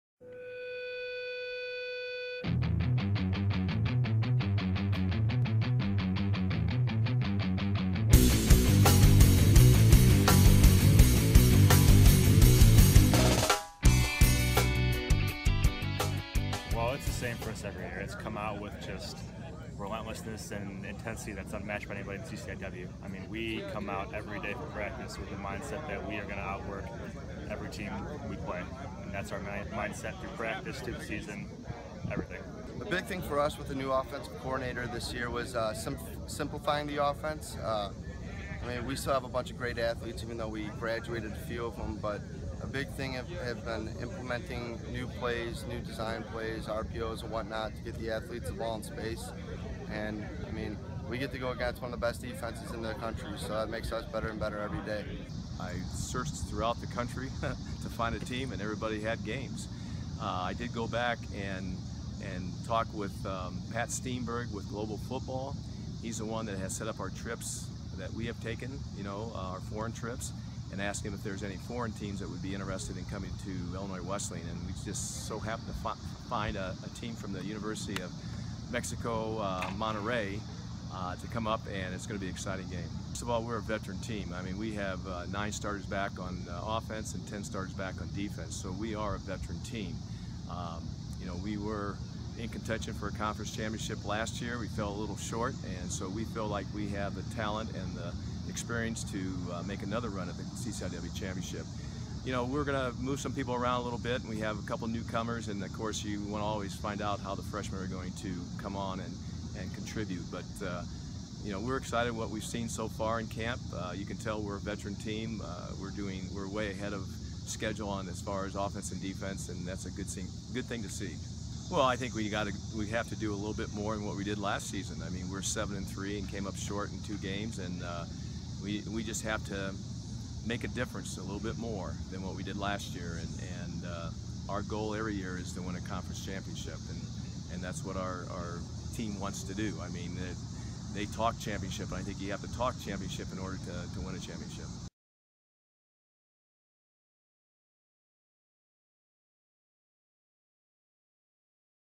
2016 CCIW Football Preview #5: Illinois Wesleyan
Illinois Wesleyan Titan senior Sean Garvey and junior Jack Warner, along with head coach Norm Eash, discuss the upcoming season.